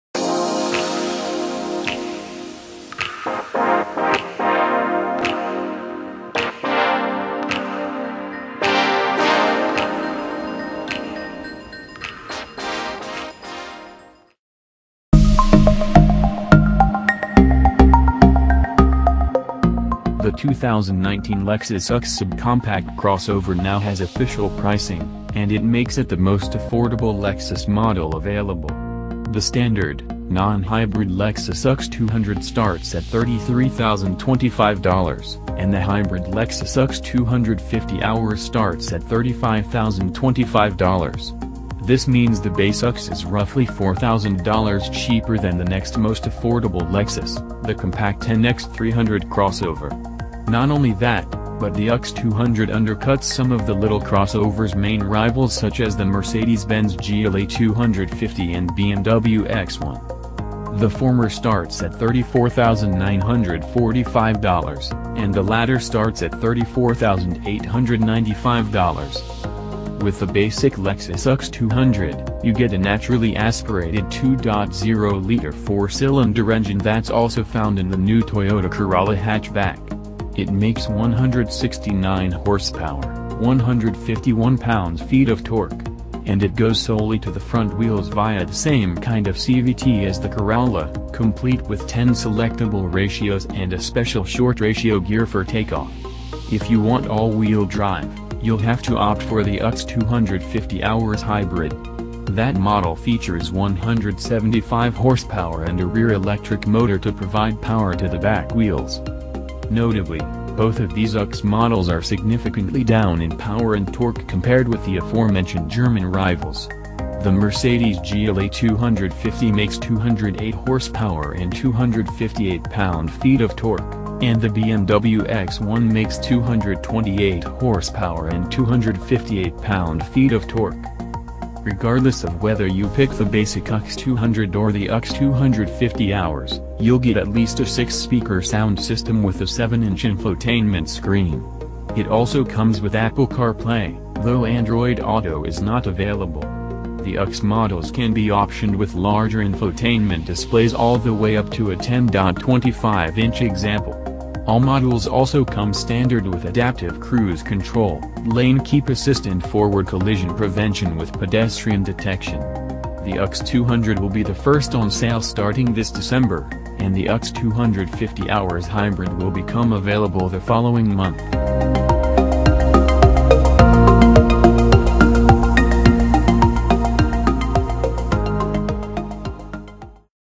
2019 Lexus UX now the most affordable Lexus
The 2019 Lexus UX subcompact crossover now has official pricing, and it makes it the most affordable Lexus model available. The standard, non-hybrid Lexus UX 200 starts at $33,025, and the hybrid Lexus UX 250h starts at $35,025. This means the base UX is roughly $4,000 cheaper than the next most affordable Lexus, the compact NX 300 crossover. Not only that, but the UX 200 undercuts some of the little crossover's main rivals such as the Mercedes-Benz GLA 250 and BMW X1. The former starts at $34,945, and the latter starts at $34,895.